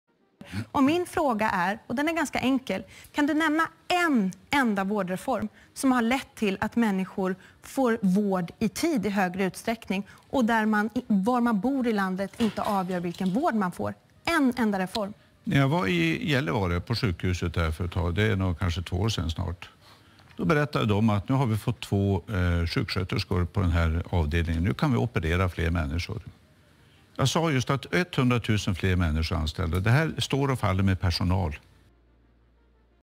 Stefan Löfvén gör bort sig...igen
Stefan Löfvén på frågan om hans vårdreform lett till arbete:

"För två år sedan anställde man 2 stycken nya på ett sjukhus i Gällivare". ?